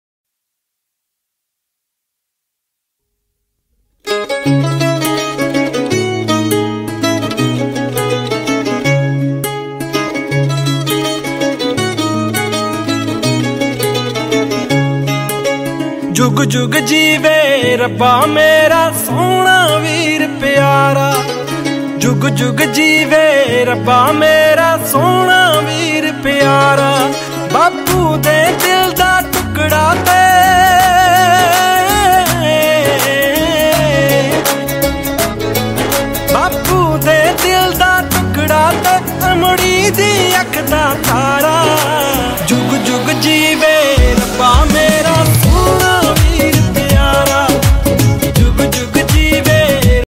Pure Kota Cotton with block prints - Office/Festival/Small function/Party wear: Perfect Rakhi Gift
Kurta : 2.5m Pure Kota Cotton printed fabric with block prints

Dupatta: 2.5 m Pure Kota Cotton printed with block prints, lace and tassels

Bottom : 2.5 m Pure Cotton

Rs 1350/-

No COD

Festival sale : Limited stock book fast